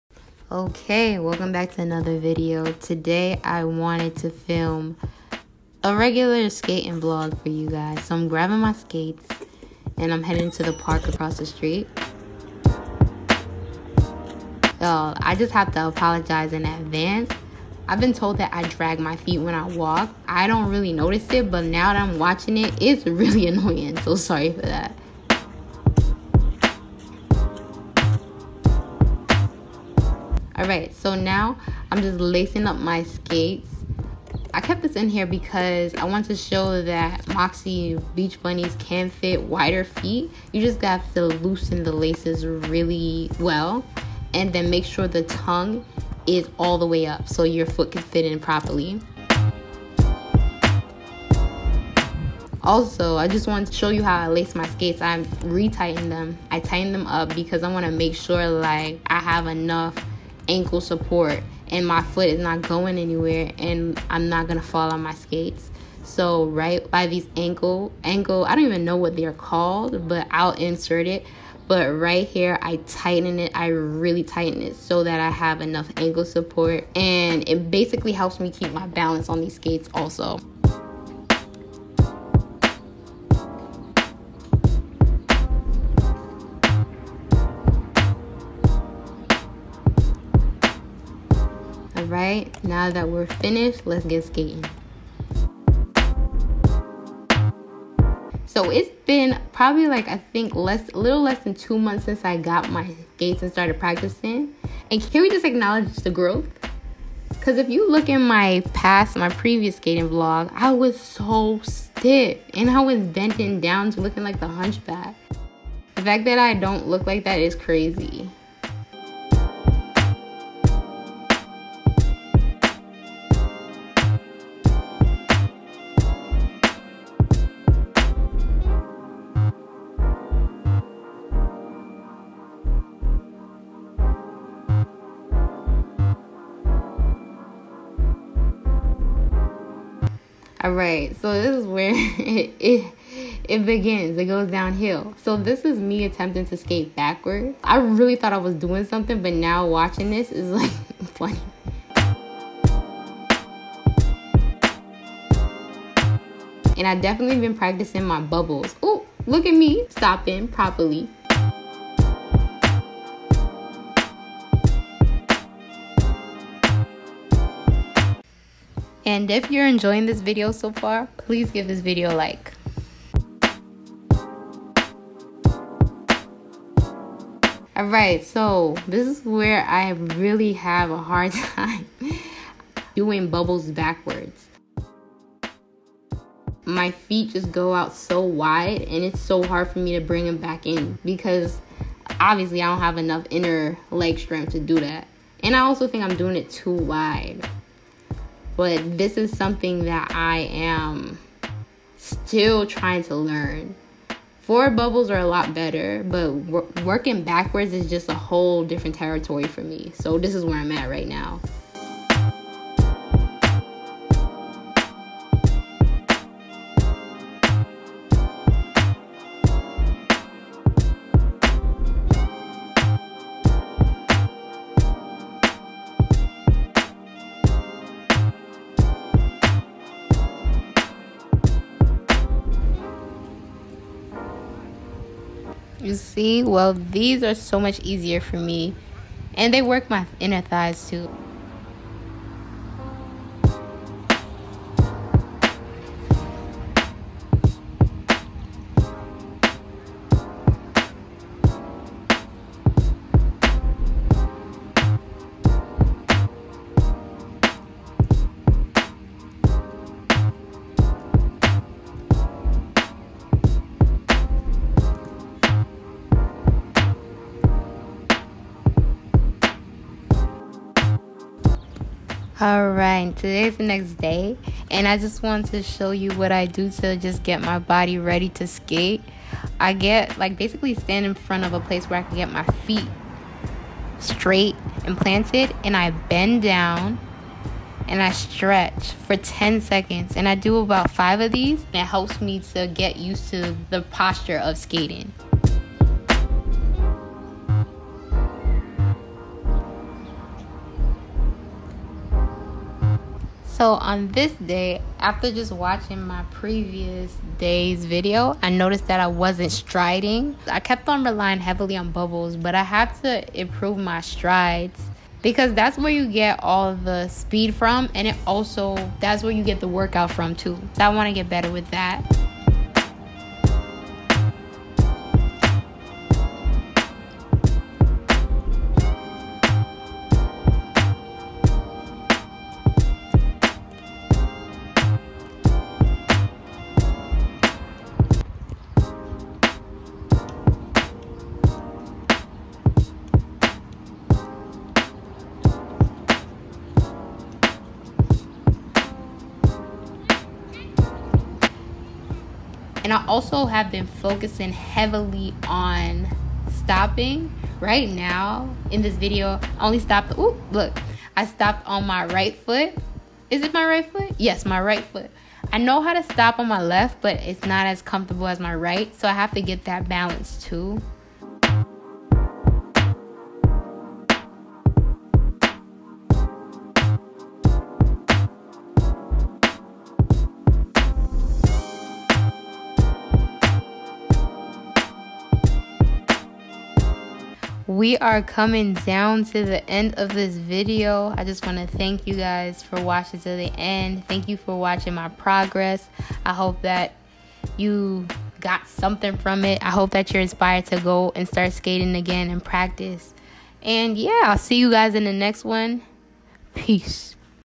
2 Month Roller Skating Progress with Moxi Beach Bunnies| Skate Vlog#2 | mlti-lyrd.
Hey Fam !
In this video I just show you where I stand on my roller skating journey two months in. I definitely recommend "The Dirty School of Skate" Youtube channel ) if you want to get the basics down pact. I contribute some of my growth to the channels tips and instructions, it's highkey a gem!

Stay Blessed & Peace ✌🏾